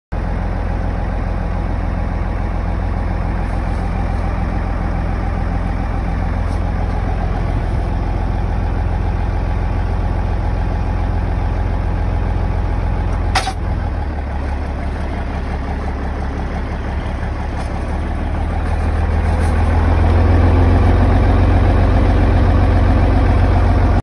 1977 JOHN DEERE 6030 For Sale
SN B313R 036607R
1977 model
8 speed synchro
20.8-38 w/ duals
New inside rubber
11.00-16 fronts like new
6 front weights
3 point
Quick hitch
PTO
2 hydraulics
5244 actual hours
Has original owners manual
This tractor has been in the family since new
New clutch about 126 hours ago
Factory John Deere cab
Air does work!  Needs charged
Does not leak or use any oil
Hours may change as owner is using this to move snow
Please call for any questions or to make an appointment to look!
This is a farmer owned tractor near Blair, NE.